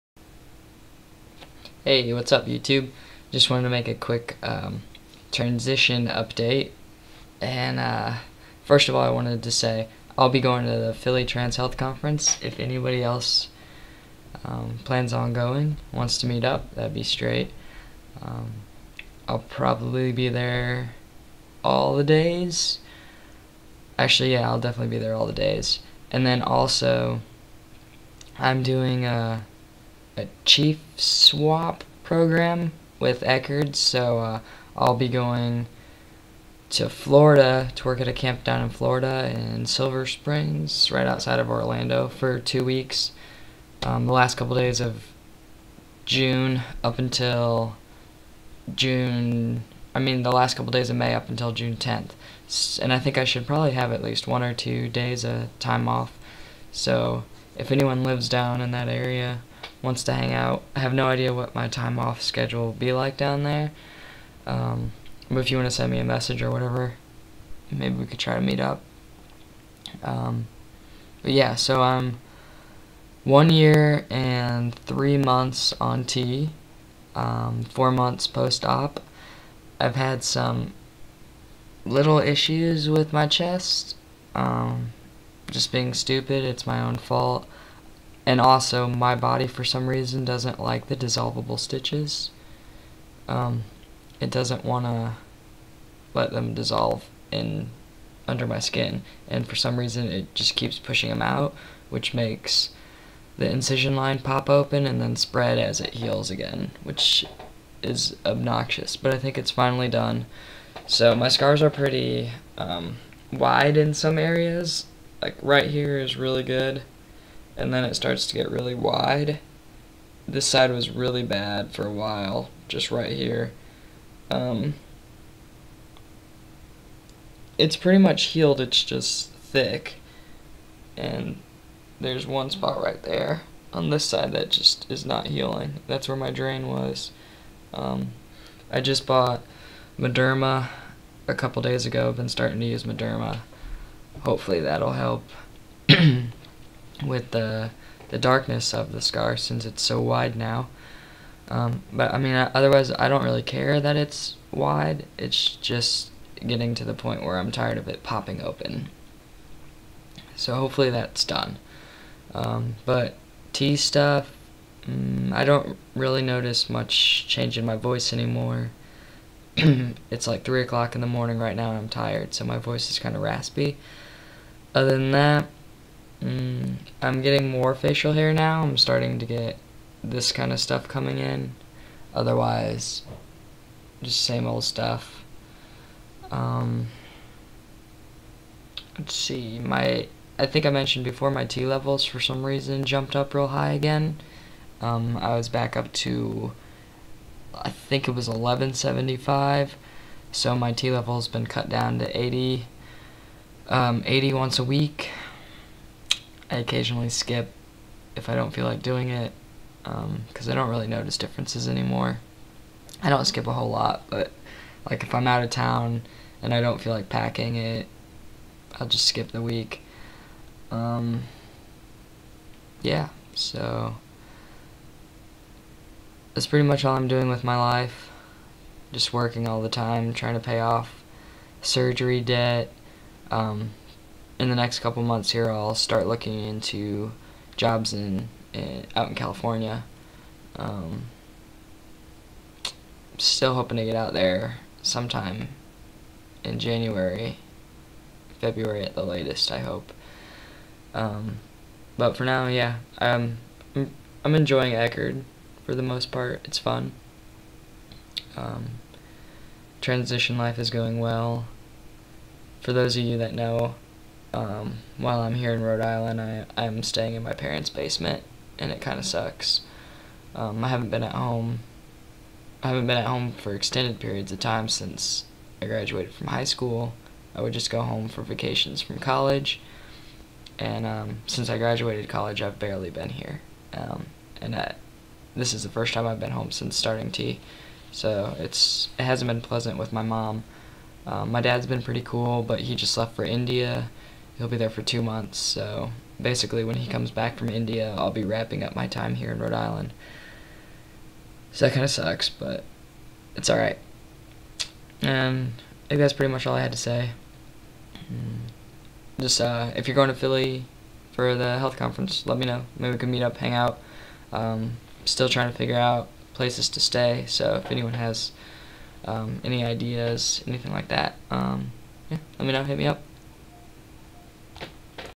Philly trans health conference anyone? 1yr3mo on T and 4 months post op
Yeah. if you're going let me know so we can chill.